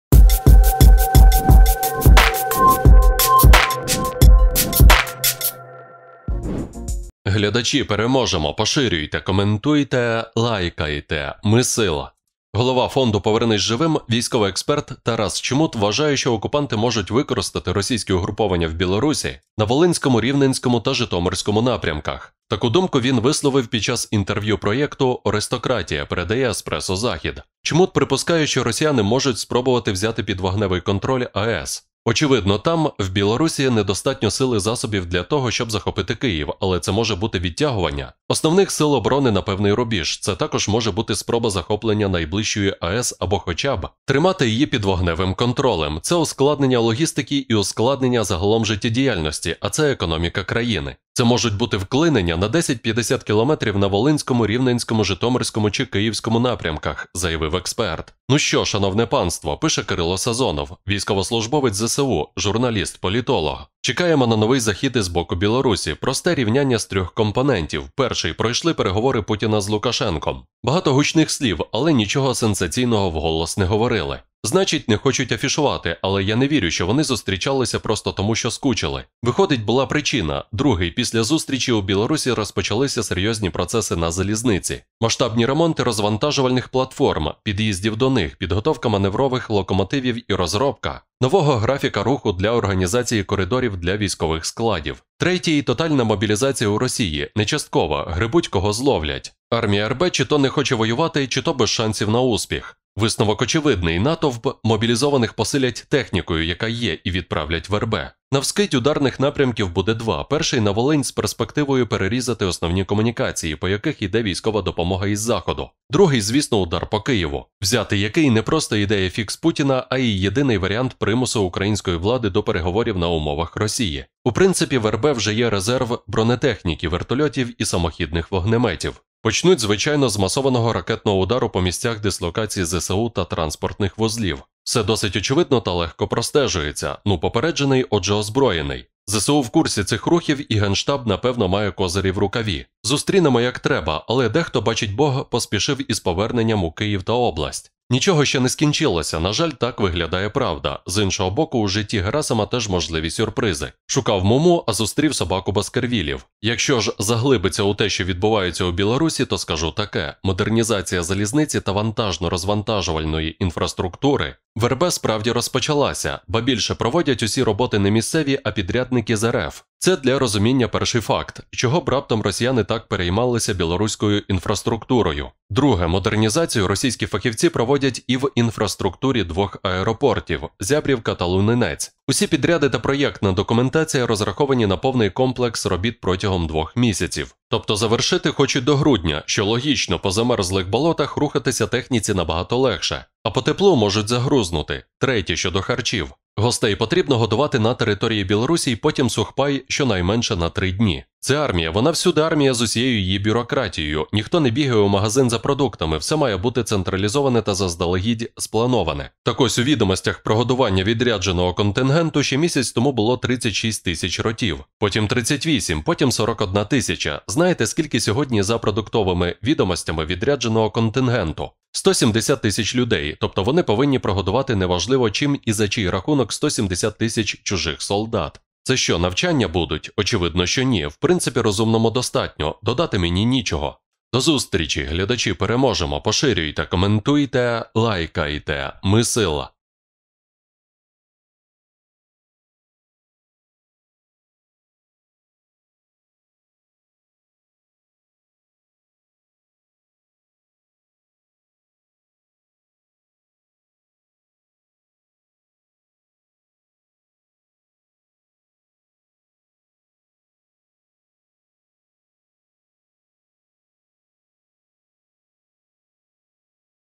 Хмельницька АЕС! Удар з Білорсі - просто на кордоні. Пізно вночі - захід. Два напрямки - ціль №1
Деталі в нашому відео.